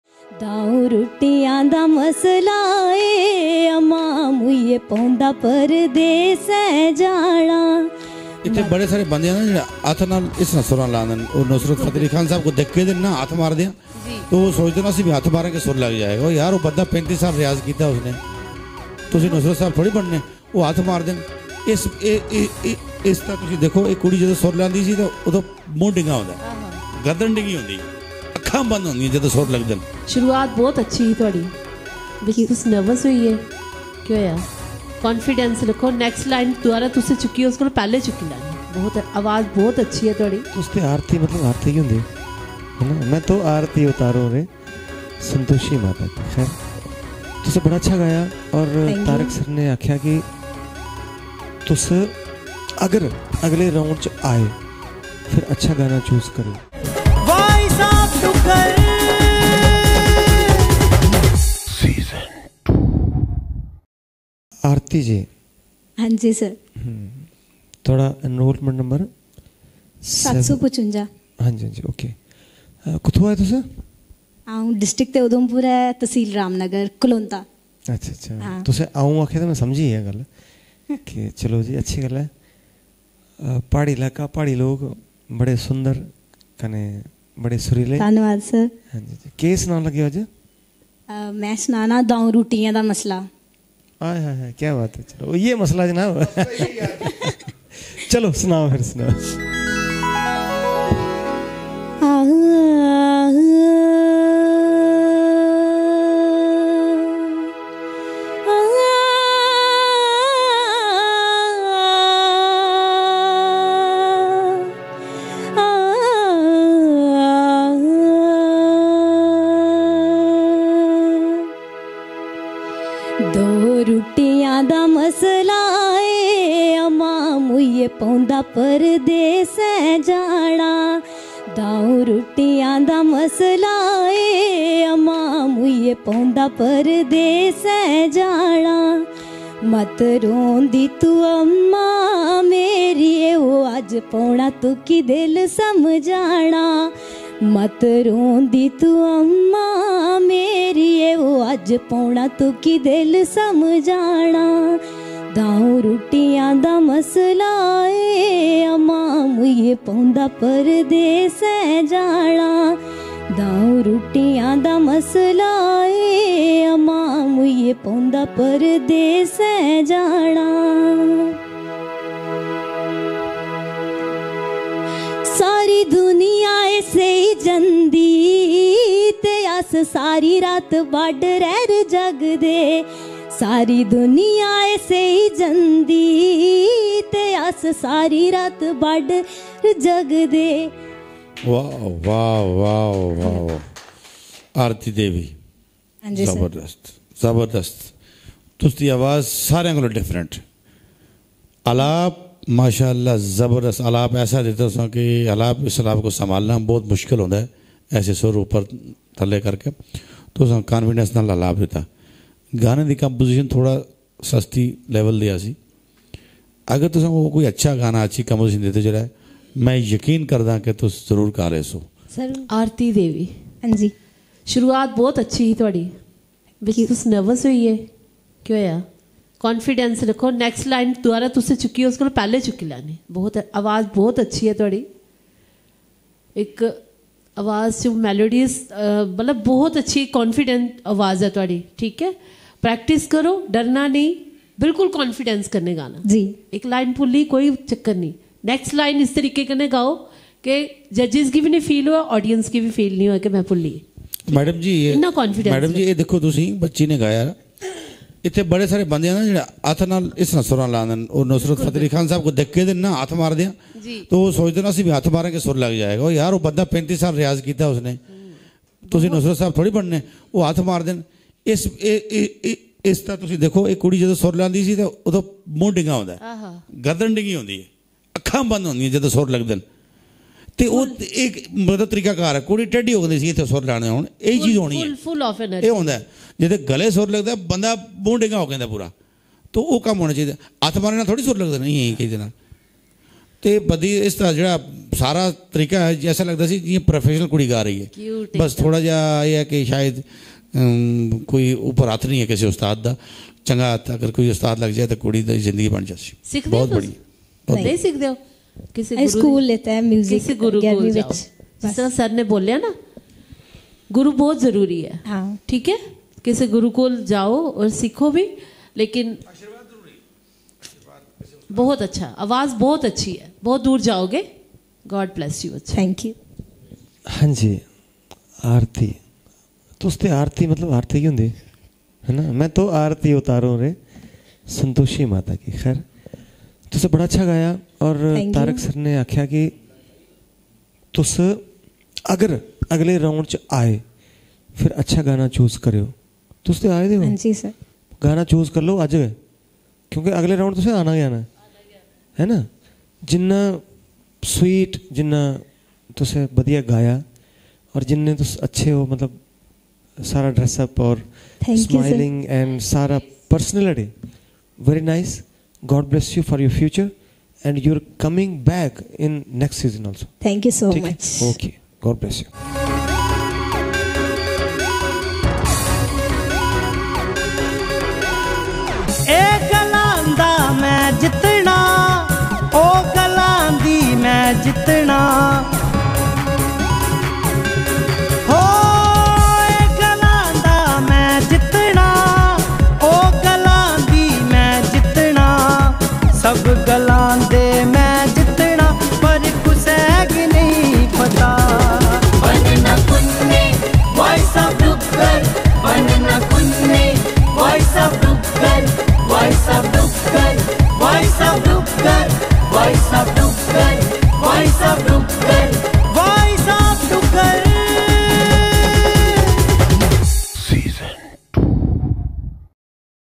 Singer Arti Bhagat| Udhampur| VOD2 Mega Audition| Her Voting Lines are open till 17/02/2024, 2:0 PM
Singer Arti Bhagat from Ramnagar Udhampur performed in VOD2 Mega Audition| Her Voting Lines are open till 17/02/2024, 2:00 PM.